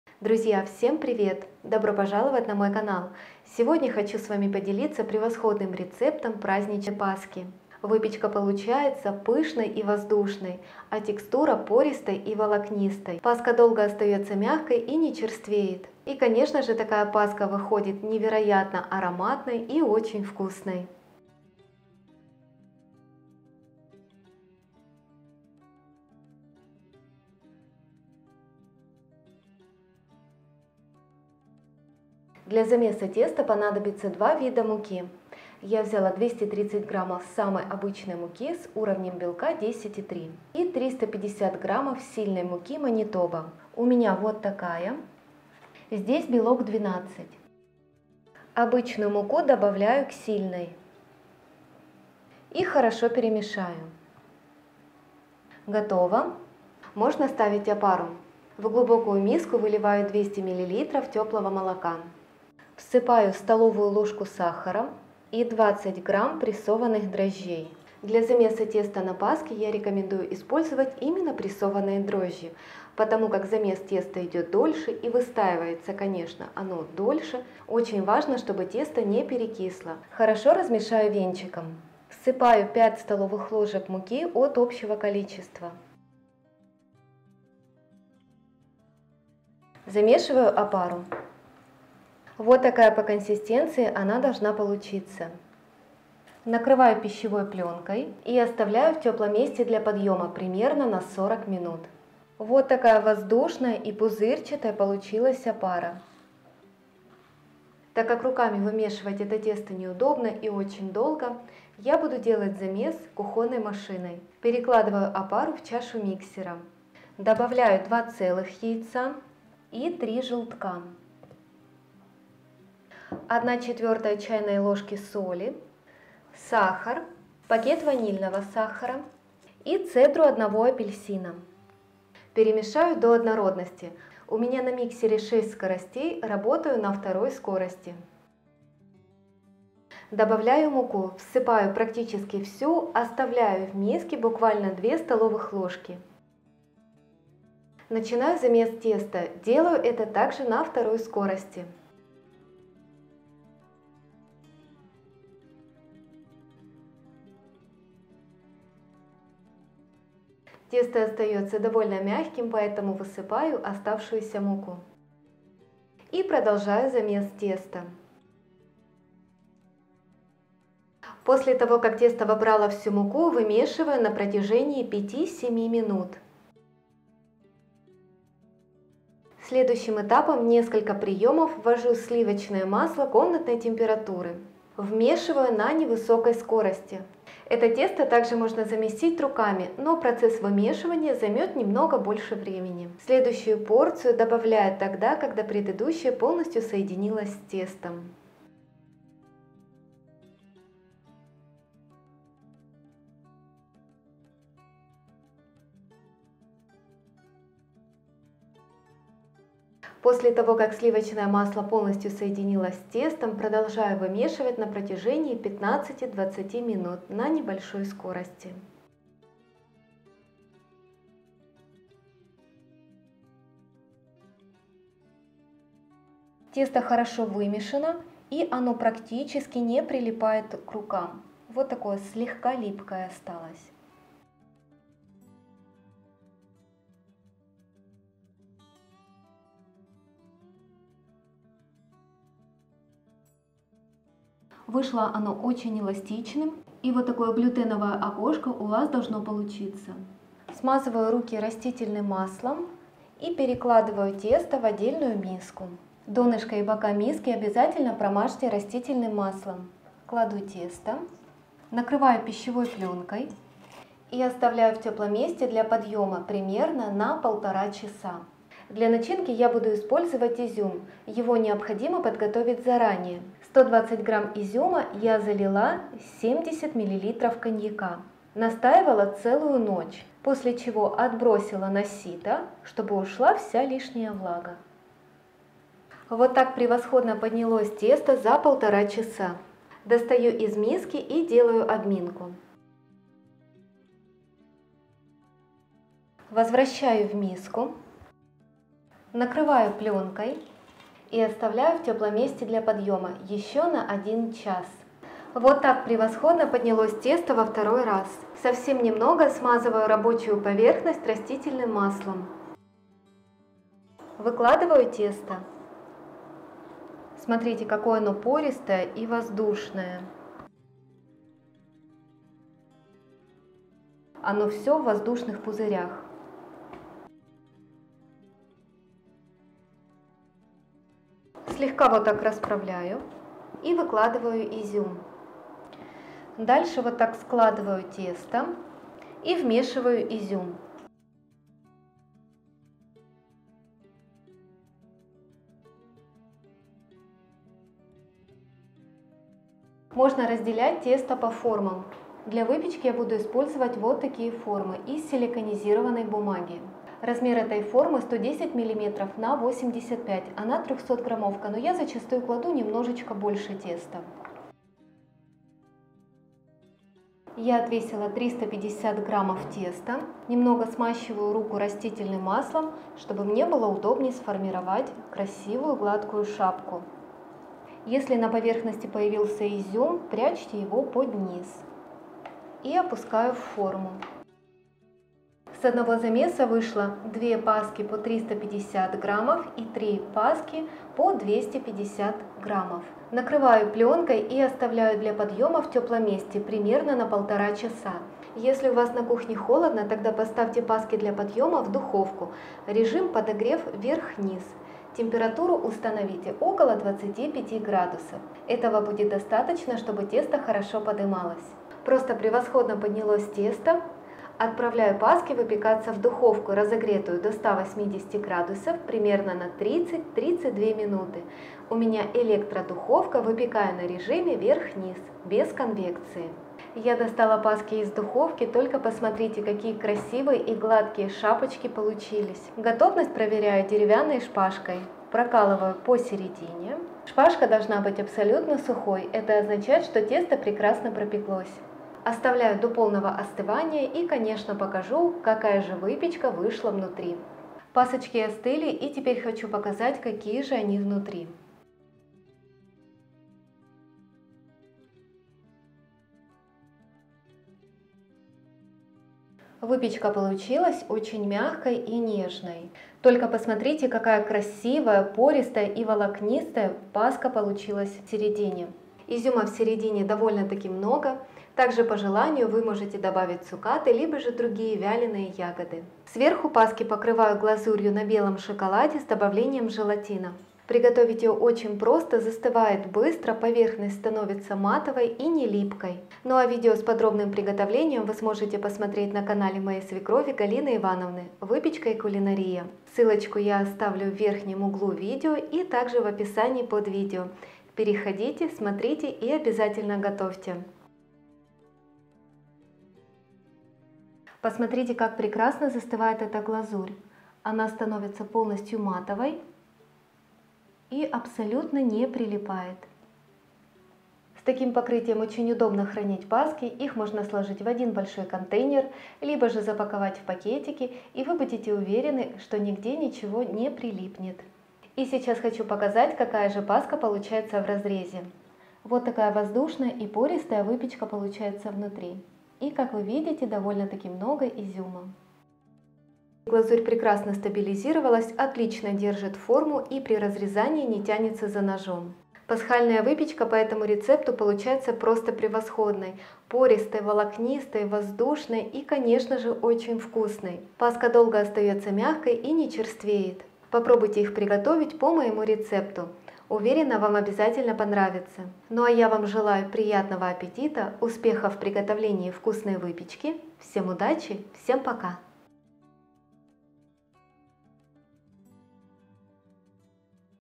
Моя БЕСПОДОБНАЯ ПАСКА.🥰 Пышное, волокнистое, сдобное тесто для пасхальной выпечки | Танюшина кухня
Хочу с вами поделится просто бесподобным рецептом паски. Выпечка выходит пышной, пористой, волокнистой и конечно же невероятно вкусной. Паски долго остаются мягкими и не черствеют.

Выход теста 1,450 кг

Так же своими результатами и рецептами Вы можете делиться в группе на FB

Друзья не забывайте делиться с нами вашими результатами, для меня это очень важно.

Рецепт
Опара
200 мл молока
20 гр пресованих дріжджів
1 ст ложка цукру
5 ст ложок борошна (від загальної кількості)

Для тіста
2 яйця
3 жовтки
170 гр цукру
1 пакет ванільного цукру (10 гр)
85 гр вершкового масла
1/4 ч ложки солі
Цедра одного апельсина
570-580 гр борошна (230 гр звичайного та 350 гр  сильної муки Манітоба)

Для начинки
120 гр ізюм
70 мл коньяк

Рецепт
Опара
200 мл молока
20 гр прессованных дрожжей
1 ст ложка сахара
5 ст ложек муки (от общего количества)

Для теста
2 яйца
3 желтка
170 гр сахара
1 пакет ванильного сахара (10 гр)
85 гр сливочного масла
1/4 ч ложки соли
Цедра одного апельсина
570-580 гр муки ( 230 гр обычной и 350 гр гр сильной муки Манитоба)

Для начинки
120 гр изюм
70 мл коньяк